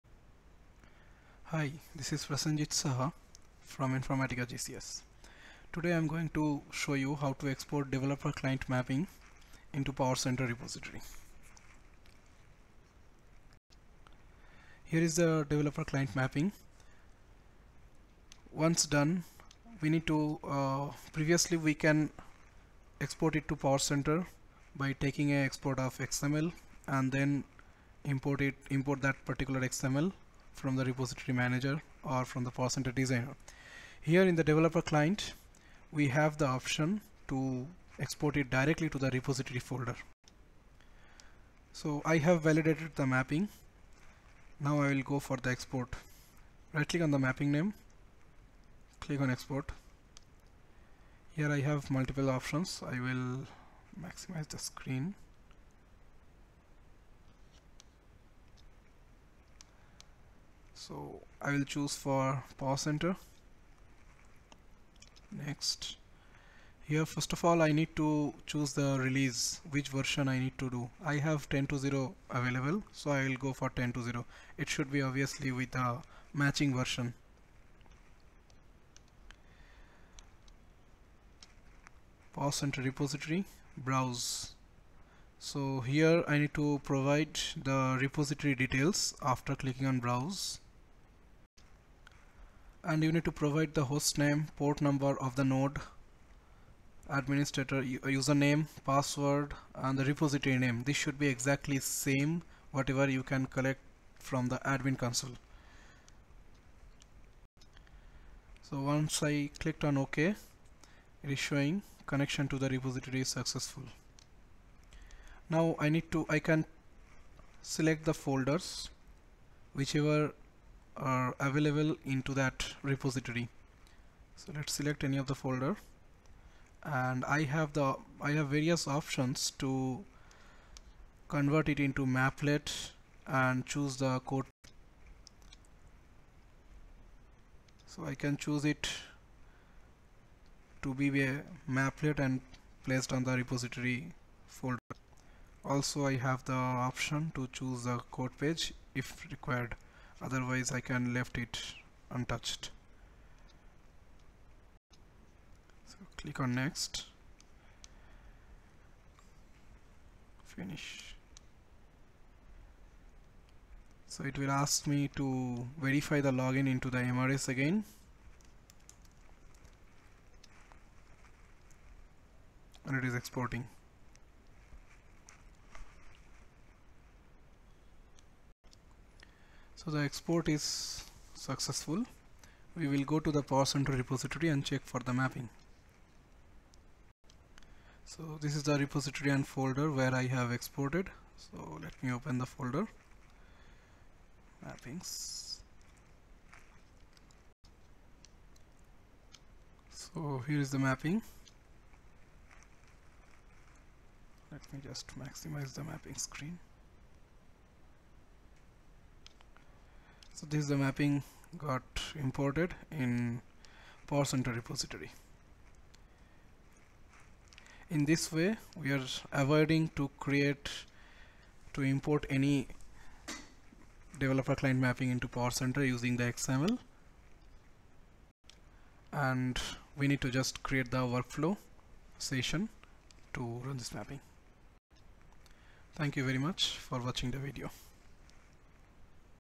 How To: Export Developer Client Mapping to PowerCenter
This video explains how to export Developer Client Mapping to PowerCenter.